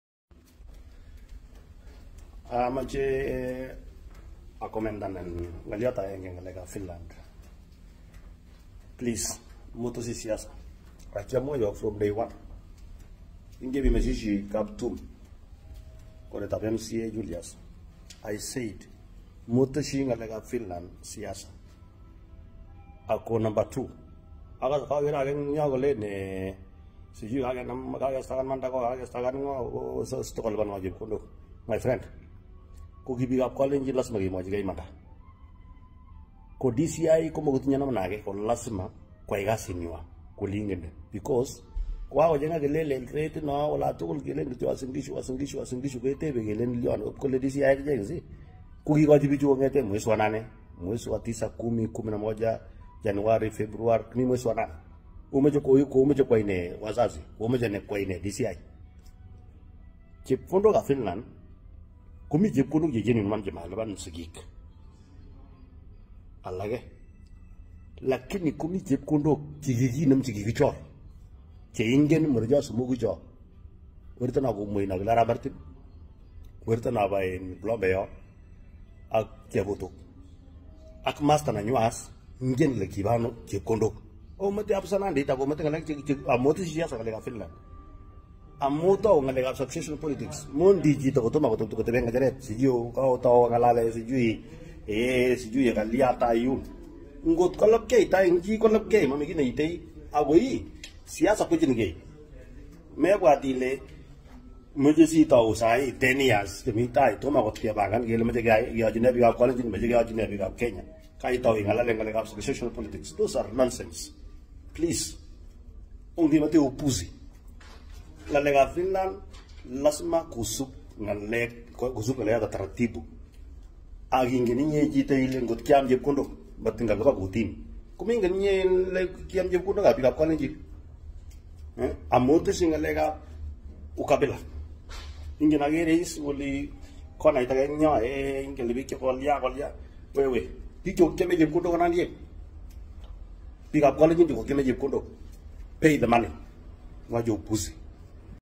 🙊 KAPSERET MP HON. OSCAR SUDI FINALLY SPEAK OUT ON THE UASIN GISHU FINLAND SCAM
Video from Commissar Ellie